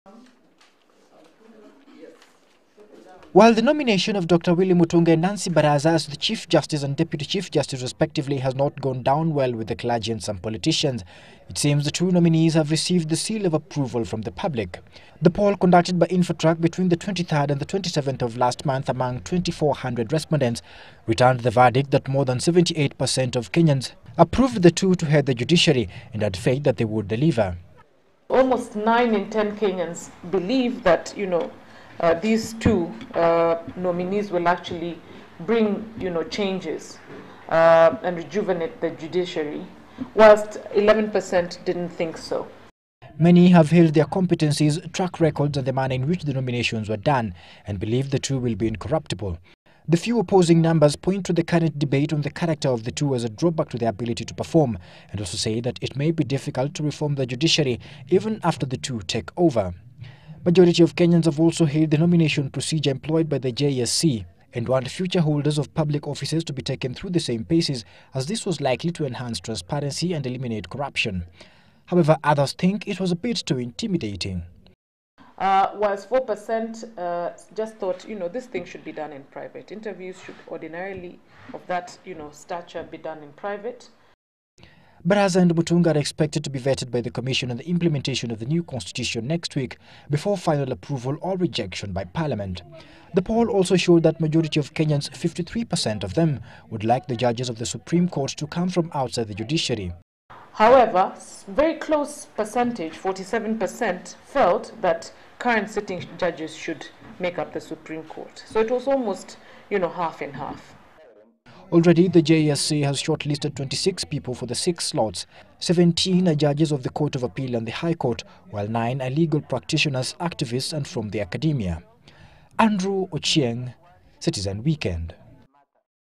Opinion Poll Over Nominees
Just after the Judicial Service Commission overlooked the Kenyan bench and nominated lawyers for positions of Chief Justice and Deputy Chief Justice, a poll conducted by research firm Infotrak Haris has indicated that a majority of Kenyans would also like Supreme Court Judges to be appointed from the legal fraternity, and not the current crop of judges. 53% of those polled said they preferred an outsider compared to the 47% who want members of the current Judiciary to take the posts. The survey also indicated that 78% of Kenyans approve of the nominations of Dr. Willy Mutunga and Nancy Baraza for the positions of Chief Justice and Deputy Chief Justice respectively.  Andrew Ochieng has details of the findings of that survey.